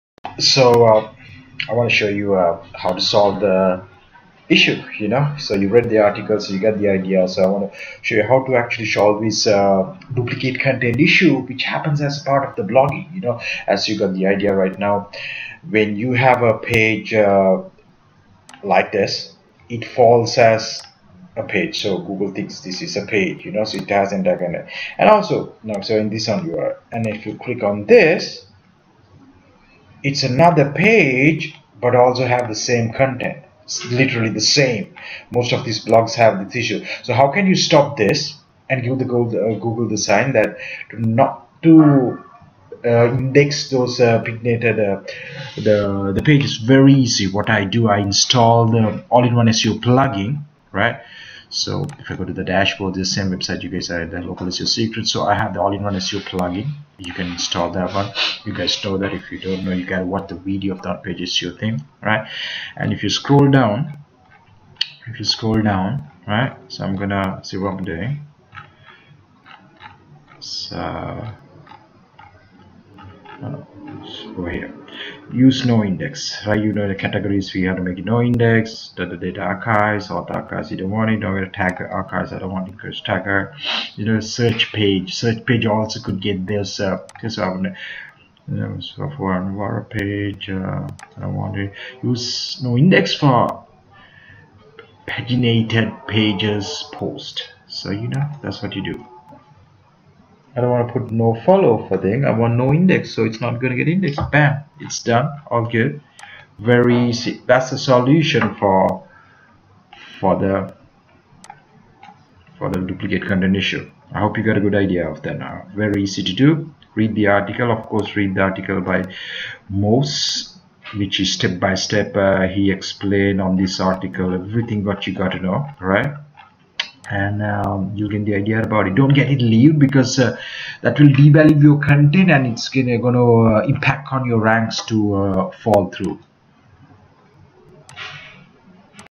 How to solve the Pagination & Duplicate Content Issue
In this video, you will learn How to solve the Pagination & Duplicate Content Issue as explained

You can get the Local SEO Secrets to learn more.